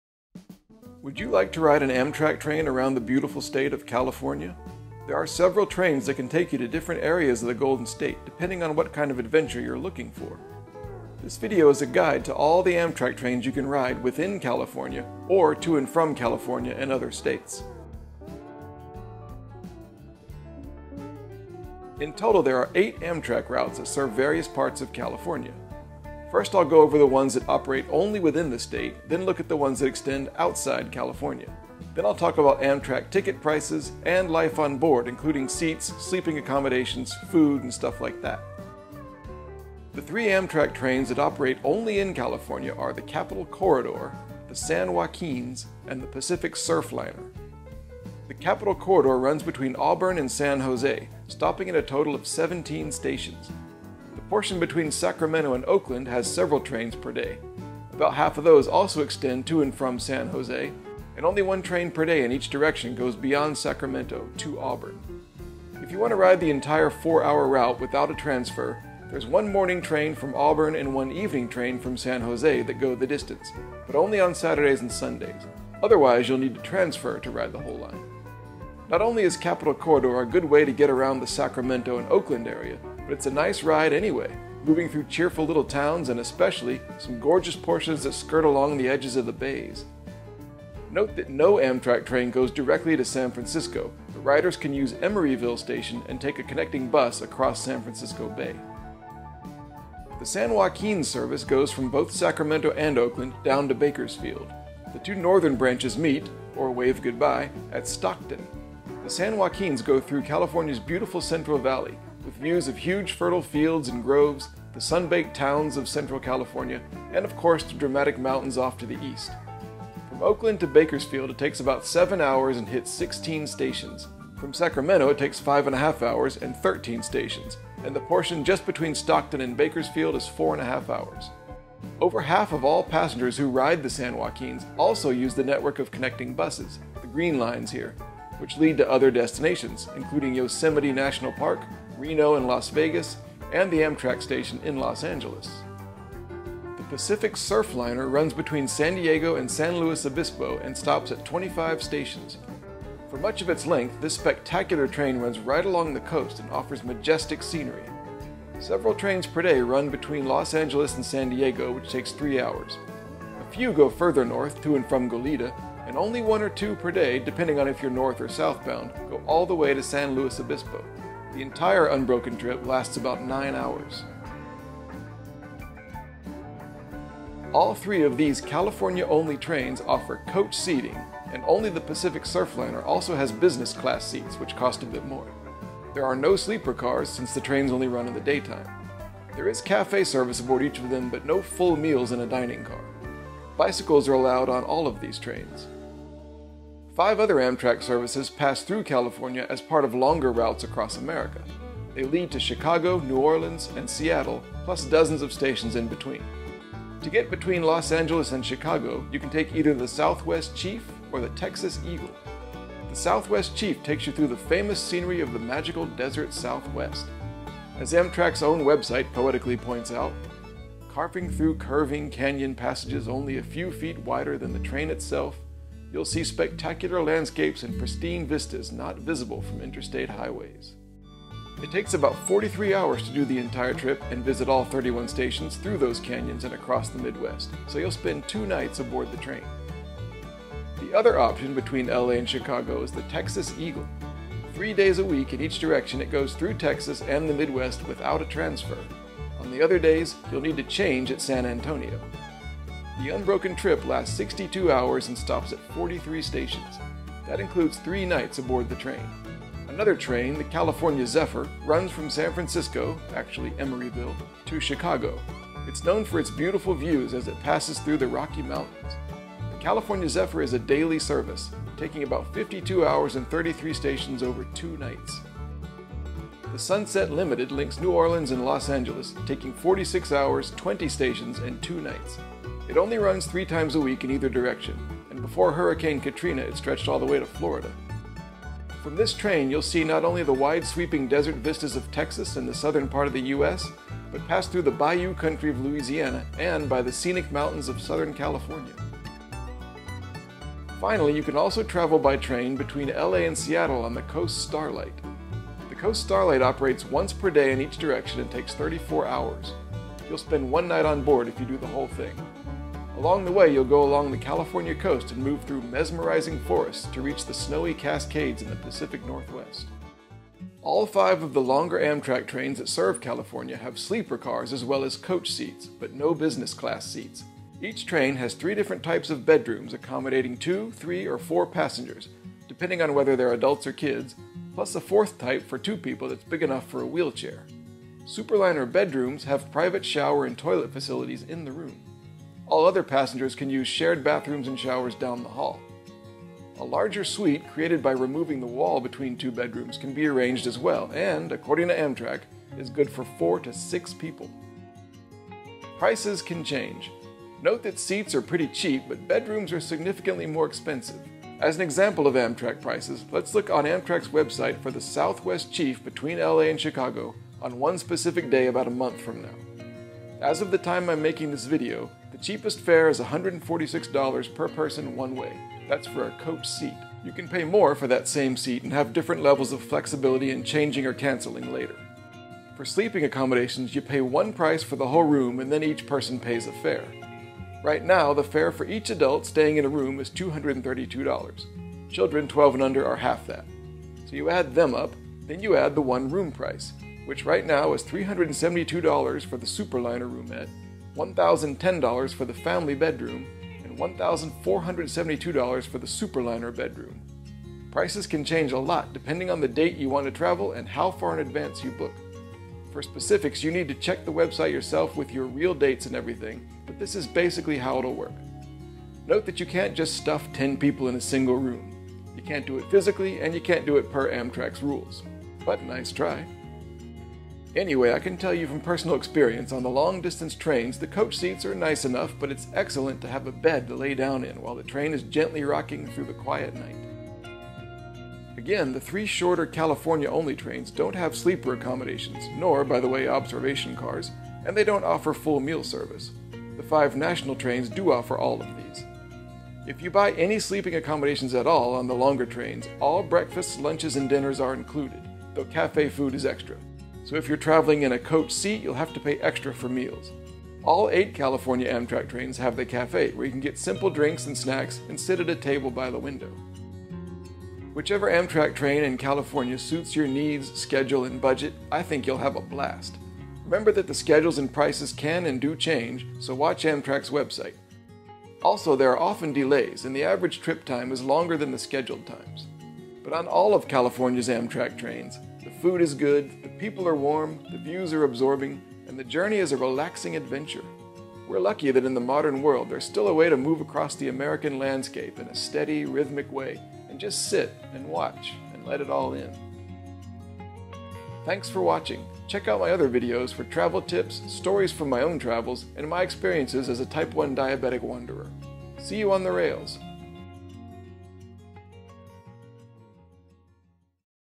Amtrak California routes, maps, prices, and seat information
A comprehensive guide to the Amtrak trains of California including maps, seats, sleeper cars, food, and ticket price info. Find out what Amtrak in California is really like!

0:00 California Amtrak intro
0:48 Amtrak Capital Corridor in California
1:39 Amtrak San Joaquins in California
2:26 Amtrak Pacific Surfliner in California
2:56 Seat types on California trains
3:32 Amtrak Southwest Chief between California and Chicago
4:00 Amtrak Texas Eagle between California and Chicago
4:20 Amtrak California Zephyr between California and Chicago
4:36 Amtrak Sunset Limited between California and New Orleans
5:00 Amtrak Coast Starlight between California and Seattle
5:20 Bedrooms on California Amtrak trains
5:55 Example prices on Amtrak California trains
7:29 Amtrak food in California
7:49 California Amtrak outro

See you on the rails!